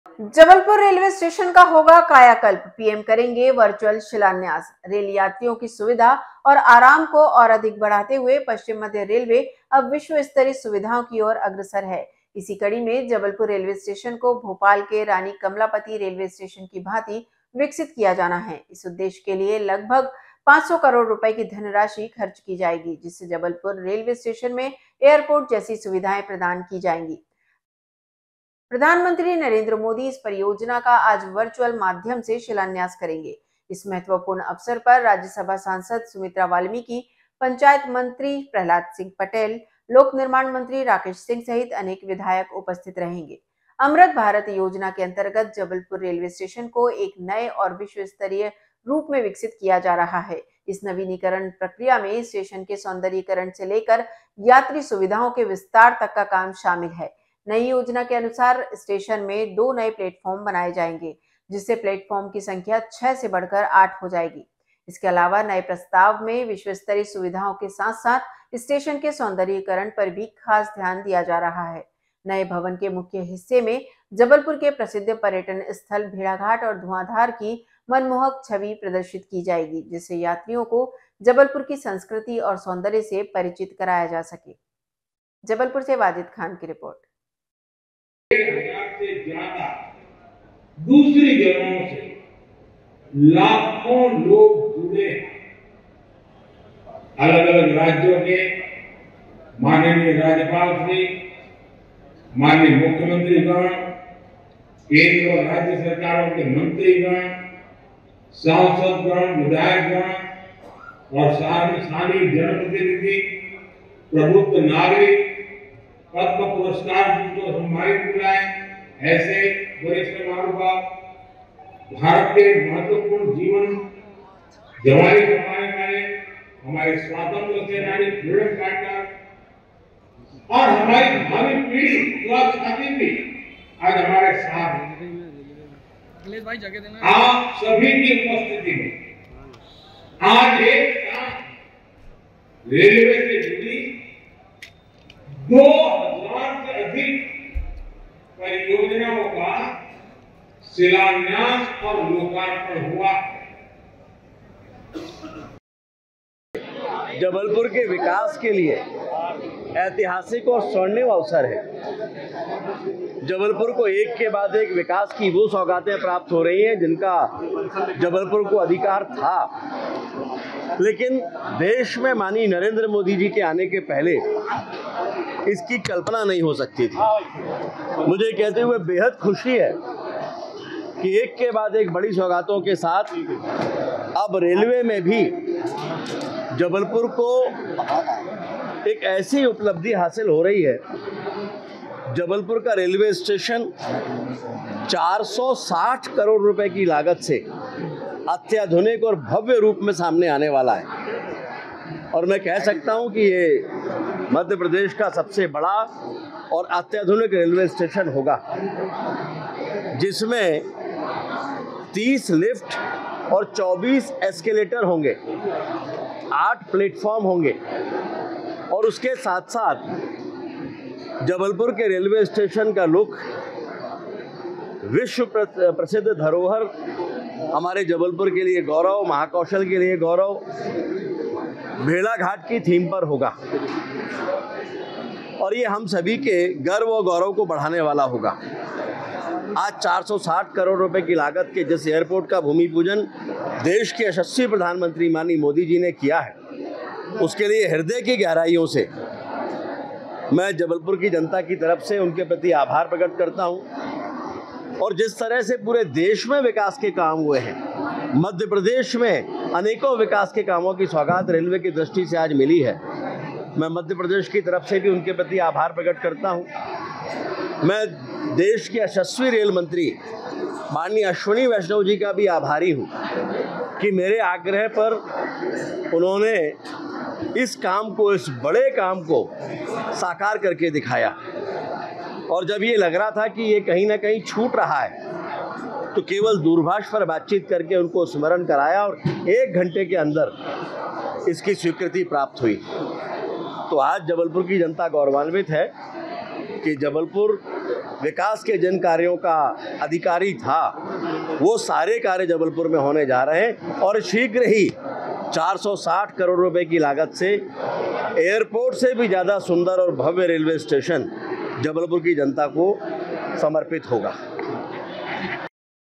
जबलपुर रेलवे स्टेशन का होगा कायाकल्प, पीएम करेंगे वर्चुअल शिलान्यास
जबलपुर: रेल यात्रियों की सुविधा और आराम को और अधिक बढ़ाते हुए, पश्चिम मध्य रेलवे अब विश्व स्तरीय सुविधाओं की ओर अग्रसर है। इसी कड़ी में, जबलपुर रेलवे स्टेशन को भोपाल के रानी कमलापति रेलवे स्टेशन की भांति विकसित किया जाना है। इस उद्देश्य के लिए लगभग 500 करोड़ रुपए की धनराशि खर्च की जाएगी, जिससे जबलपुर रेलवे स्टेशन में एयरपोर्ट जैसी सुविधाएं प्रदान की जाएंगी।

प्रधानमंत्री नरेंद्र मोदी इस परियोजना का आज वर्चुअल माध्यम से शिलान्यास करेंगे। इस महत्वपूर्ण अवसर पर, राज्यसभा सांसद सुमित्रा वाल्मीकि, पंचायत मंत्री प्रहलाद सिंह पटेल, लोक निर्माण मंत्री राकेश सिंह सहित अनेक विधायक उपस्थित रहेंगे।

अमृत भारत योजना के अंतर्गत जबलपुर रेलवे स्टेशन को एक नए और विश्वस्तरीय रूप में विकसित किया जा रहा है। इस नवीकरण प्रक्रिया में स्टेशन के सौंदर्यीकरण से लेकर यात्री सुविधाओं के विस्तार तक का काम शामिल है। नई योजना के अनुसार, स्टेशन में दो नए प्लेटफॉर्म बनाए जाएंगे, जिससे प्लेटफॉर्म की संख्या 6 से बढ़कर 8 हो जाएगी।

इसके अलावा, नए प्रस्ताव में विश्वस्तरीय सुविधाओं

 के साथ-साथ स्टेशन के सौंदर्यीकरण पर भी खास ध्यान दिया जा रहा है। नए भवन के मुख्य हिस्से में जबलपुर के प्रसिद्ध पर्यटन स्थल भेड़ाघाट और धुआंधार की मनमोहक छवि प्रदर्शित की जाएगी, जिससे यात्रियों को जबलपुर की संस्कृति और सौंदर्य से परिचित कराया जा सके।